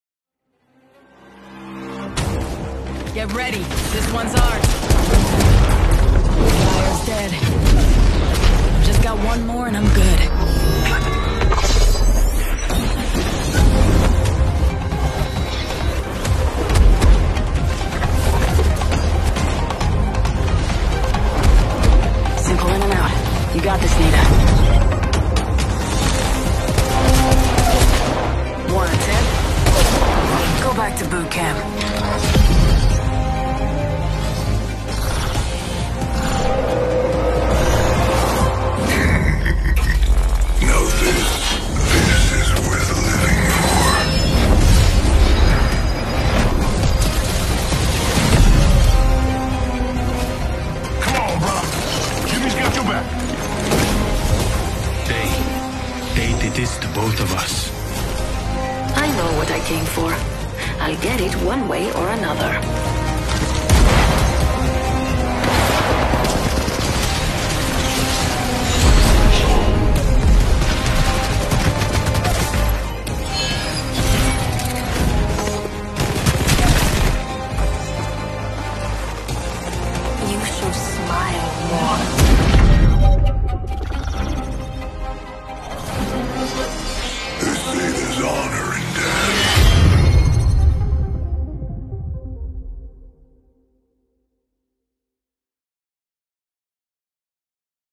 Apex Legend New Skins Coming The Genesis Collection Event | Daily Reuters
Apex Legends Season 9 collection event has finally been unveiled. The Genesis Collection Event will start on June 29, bringing back the OG Kings Canyon and World’s Edge, and more.